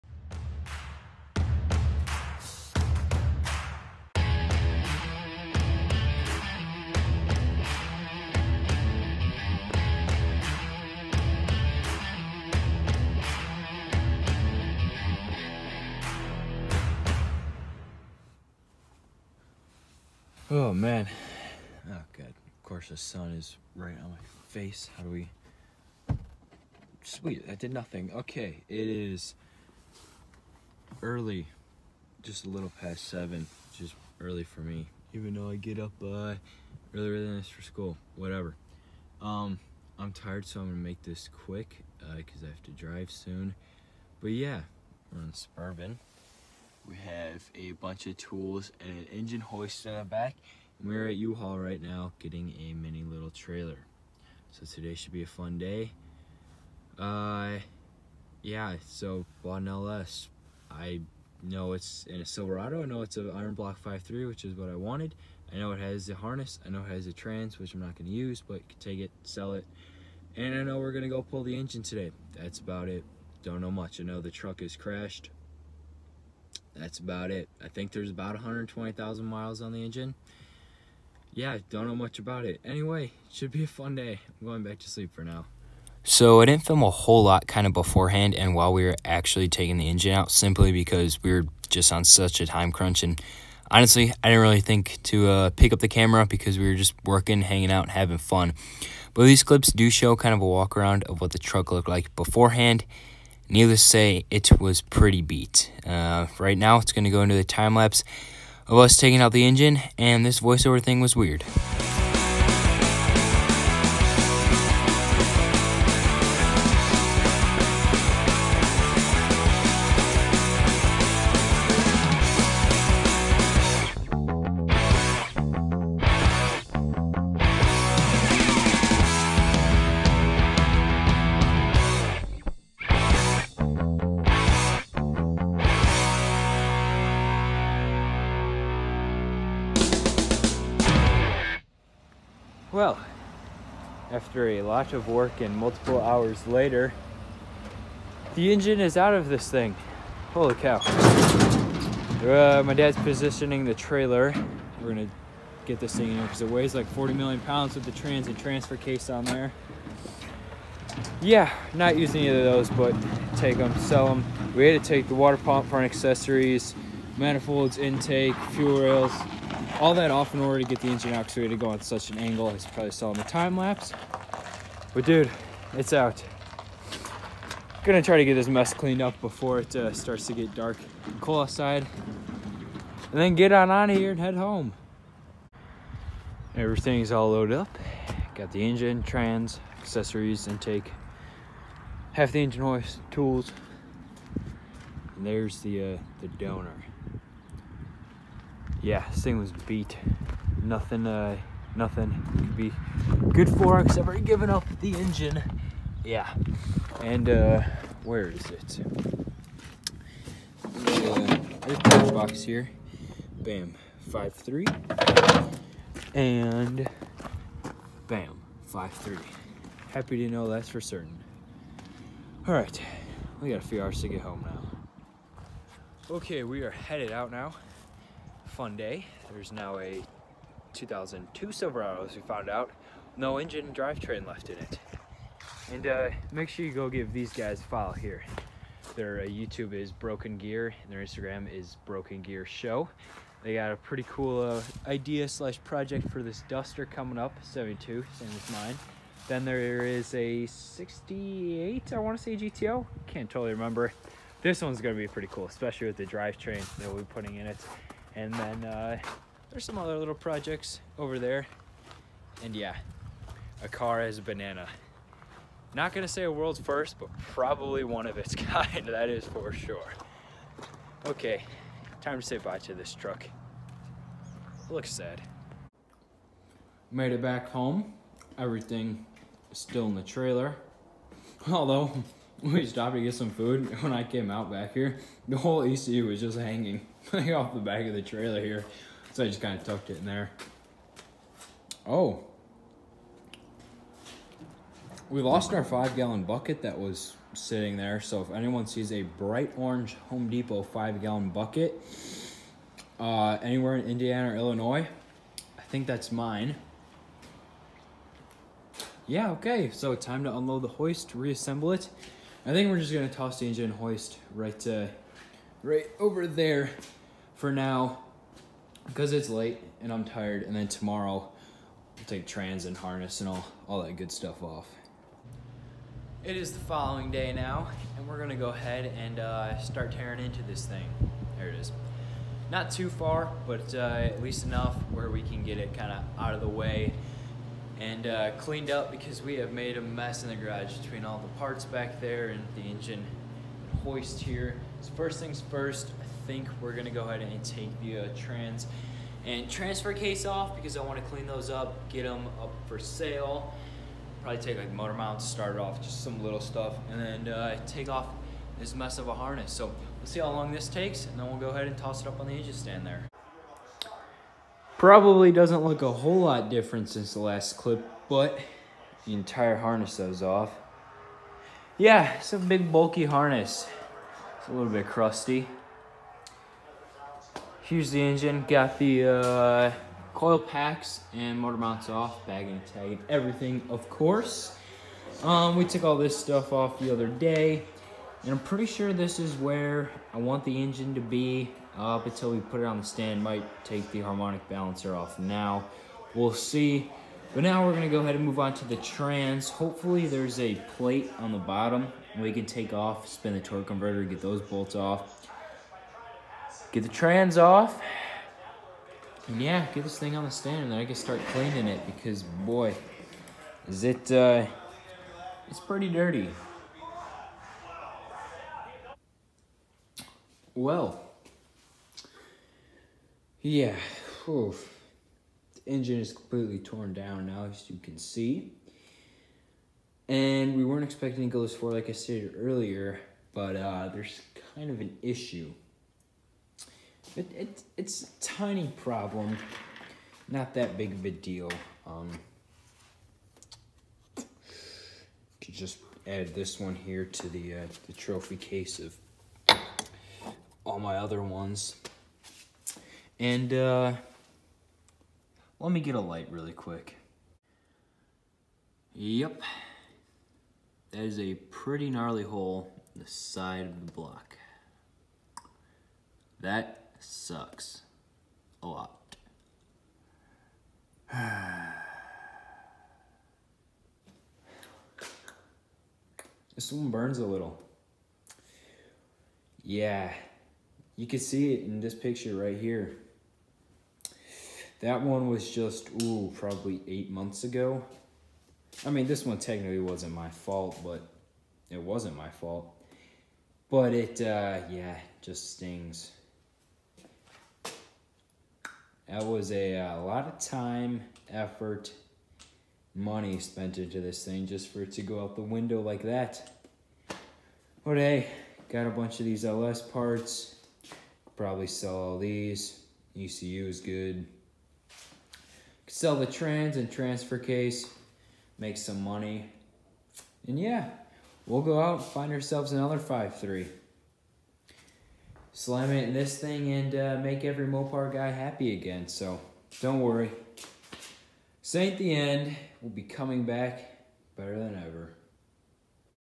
The Duster’s next engine
Although this video has an unfortunate ending, it was fun to rip this 5.3 out and tear it apart. As I said In a captioning my Instagram, the mopar gods really didn’t want me to put a Chevy engine in a Chrysler. That’s not stopping us and there should be another 5.3 coming soon and this only motivates me more to make this happen. I cannot wait to have a V8 and a 5 speed in this thing. Make sure you go give Broken Gear on youtube on Instagram!